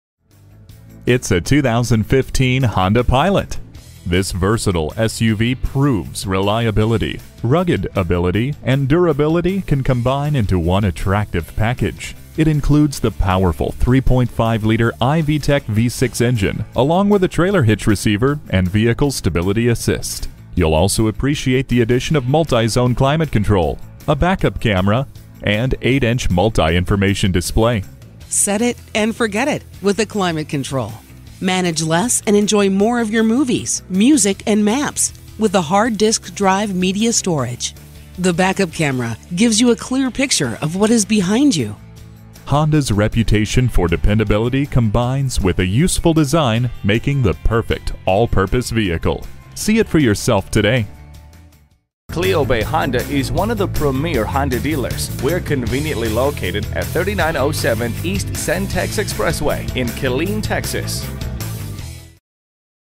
2015 Honda Pilot Killeen TX Austin, TX
Year: 2015
Make: Honda
Model: Pilot
Trim: LX FWD
Engine: 6 Cyl - 3.5 L
Transmission: Automatic
Color: Taffeta White
Interior: Gray
Mileage: 14244
Stock #: T6513A
VIN: 5FNYF3H27FB013086
New Arrival! This Honda Pilot is CERTIFIED! Bluetooth, 3rd Row Seating, Multi-Point Inspection, Popular color combo! Stability Control, ABS Brakes Front Wheel Drive Please let us help you with finding the ideal New, Used, or Certified vehicle.

Address:
3907 E Cen-Tex Expy